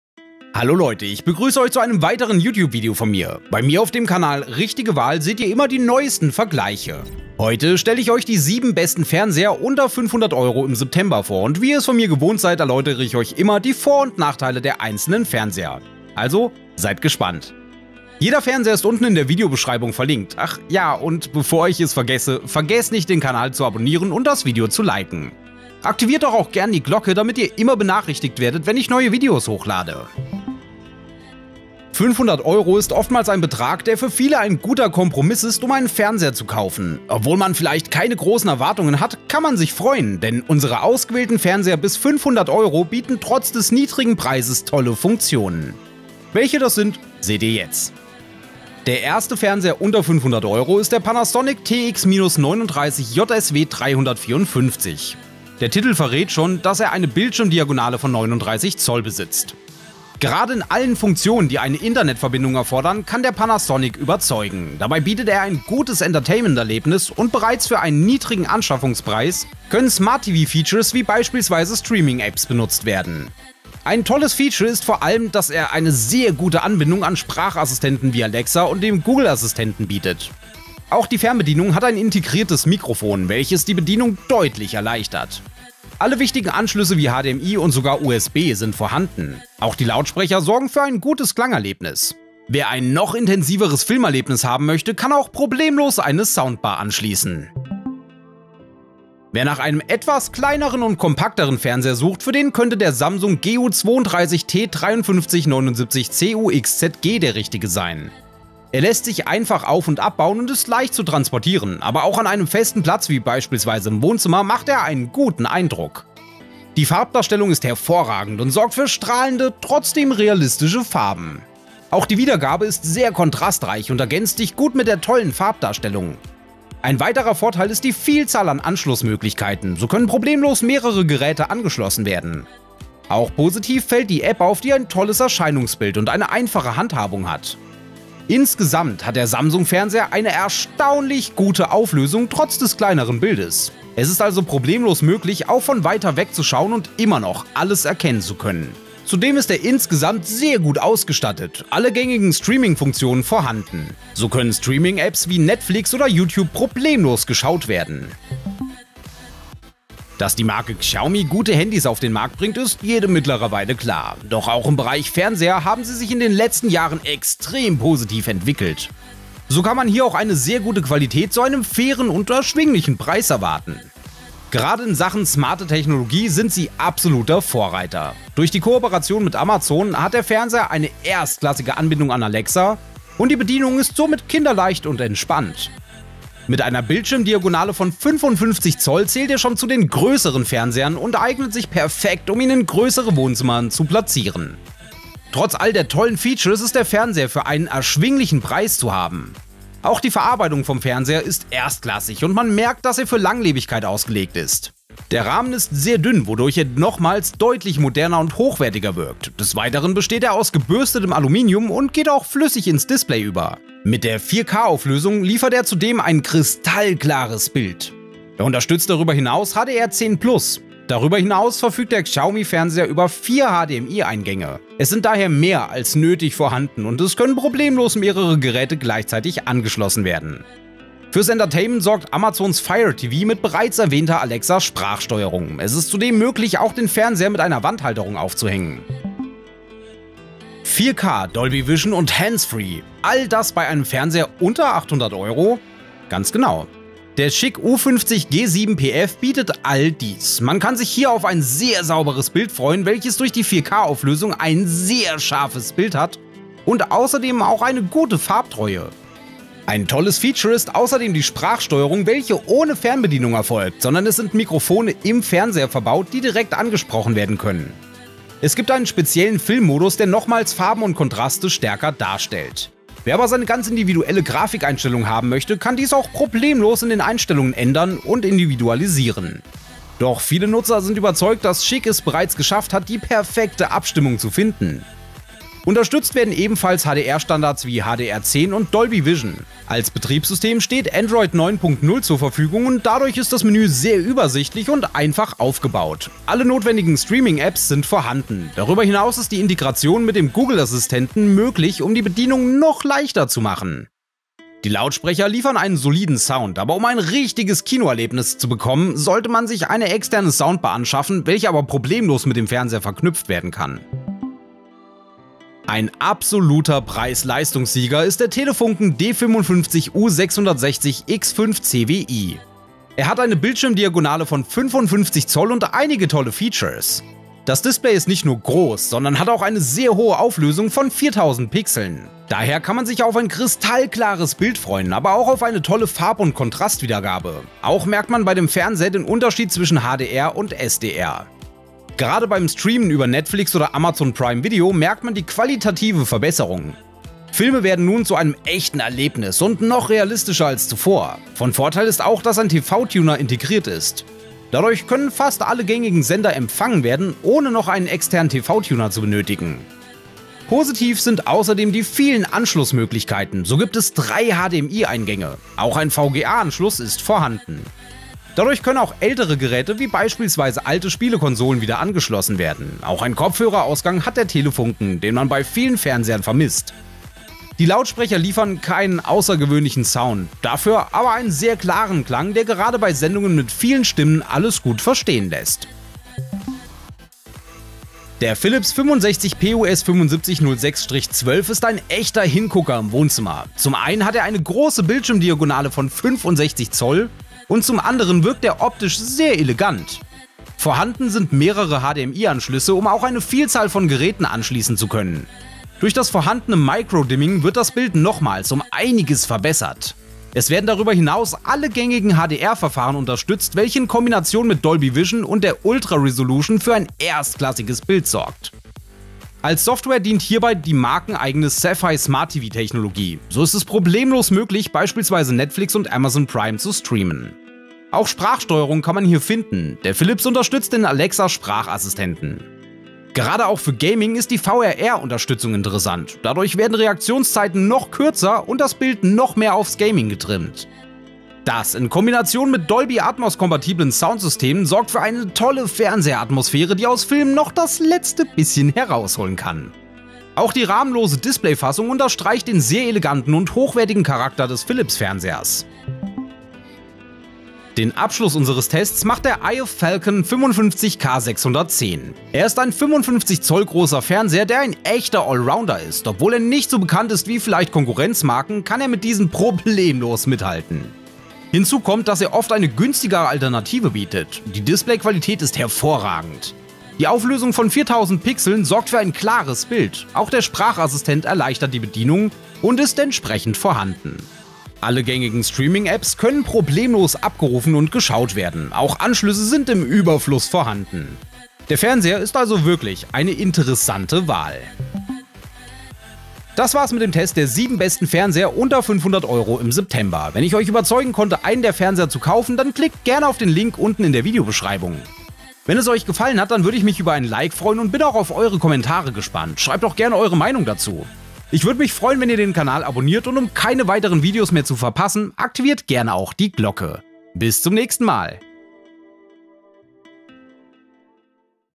TOP-7. Die besten Fernsehers bis 500€. Test & Vergleich. Dezember 2022 | Deutsch
Panasonic TX-39JSW354